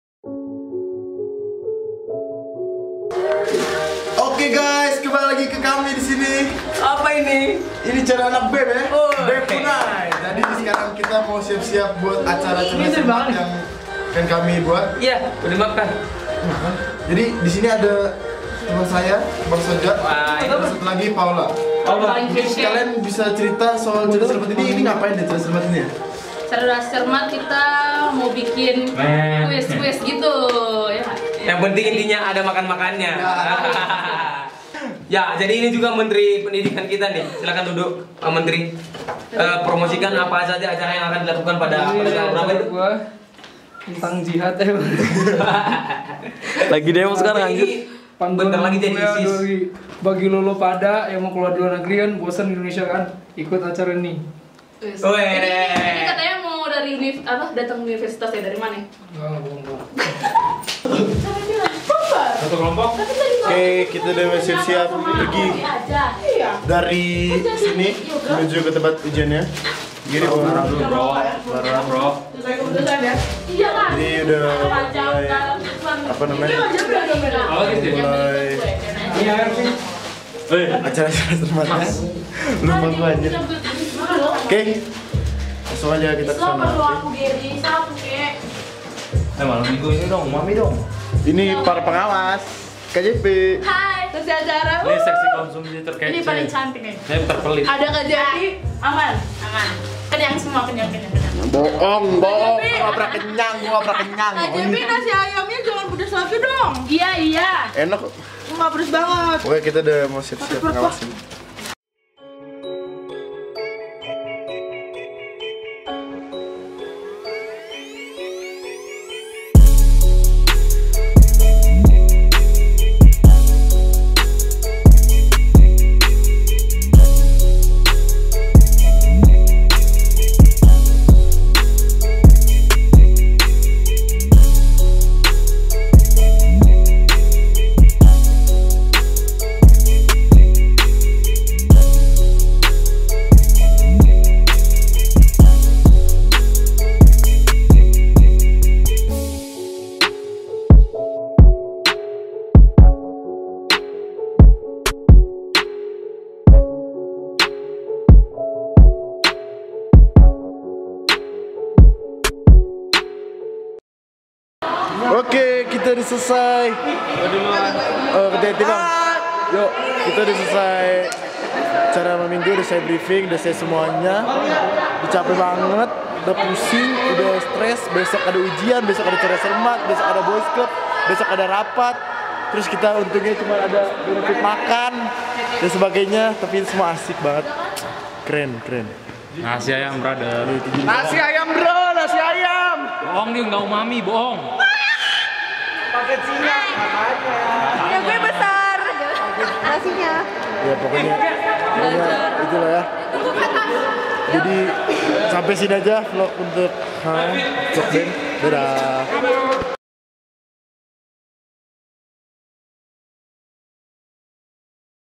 VLOG BEM UNAI Cerdas Cermat
Video ini dibuat iseng2 skalian memberi tahu apa saja kerja dari anak BEM UNAI.